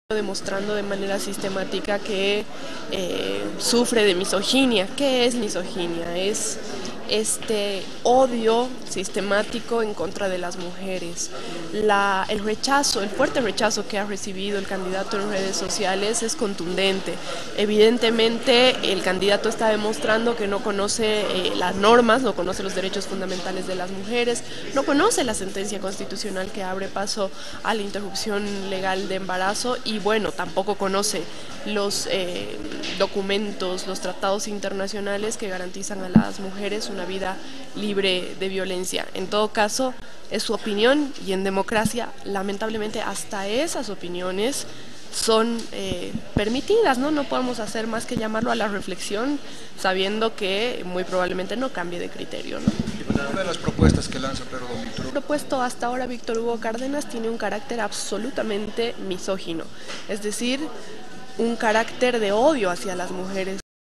07/06/19 VALERA SILVA- DIP. MAS.
SEÑALO QUE EL CANDIDATO VICTOR HUGO CARDENAS ES UN MISÓGENO PORQUE SE VE SU REPUDIO HACIA LAS MUJERES TRAS DIFERENTES CUESTIONAMIENTOS Y PROPUESTAS QUE ESTARÍA DANDO A CONOCER POR EJEMPLO EN EL TEMA DEL ABORTO.